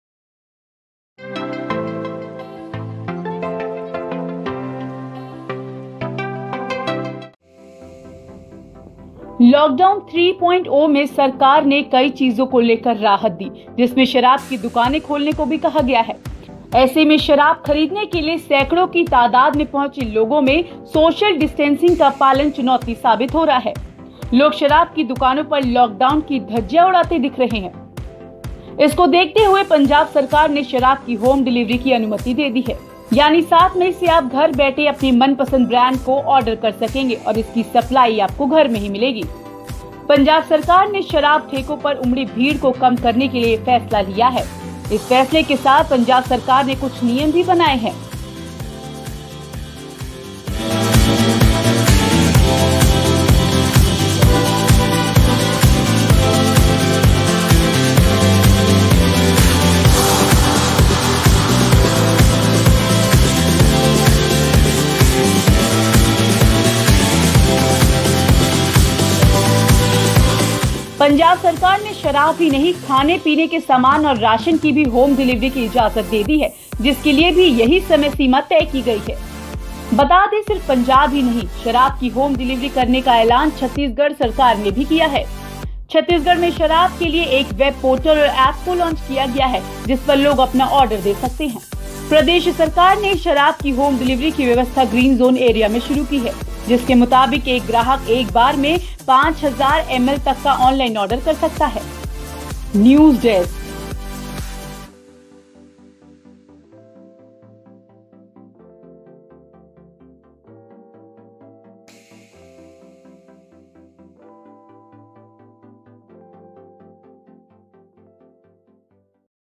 Lockdown में ये राज्य करेगा शराब की Home Delivery, राशन की चीजें भी घर पहुंचाई जाएंगी
LOCKDOWN 3.0 में सरकार ने कई चीज़ों को लेकर राहत दी जिसमें शराब की दुकान खोलने को भी कहा है..पंजाब सरकार ने शराब की होम डिलीवरी की अनुमति दे दी है...यानी 7 मई से से आप घर बैठे शराब के मनपसंद ब्रांड को ऑर्डर कर सकेंगे और इसकी सप्लाई आपको घर में मिलेगी।